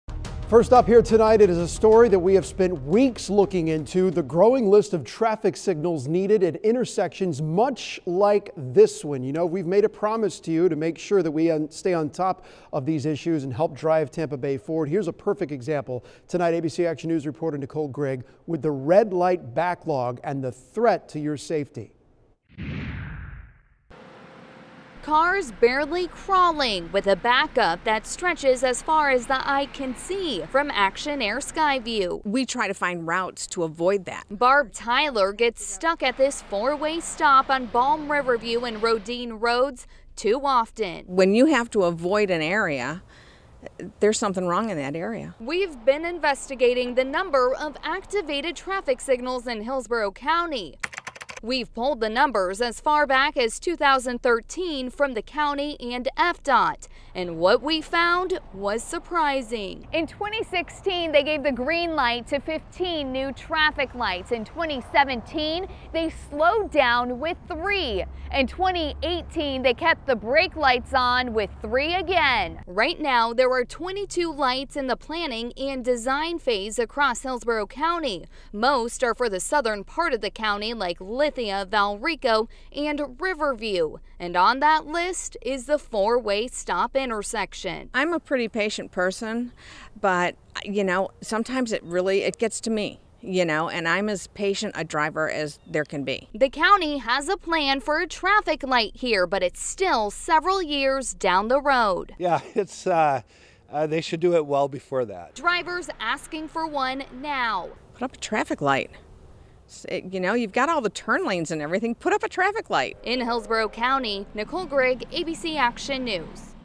Hillsborough County has a long list of traffic signals that need to go in
ABC Action News is looking into the number of traffic signals that need to be installed on roads across Hillsborough County, especially in the southern part of the county.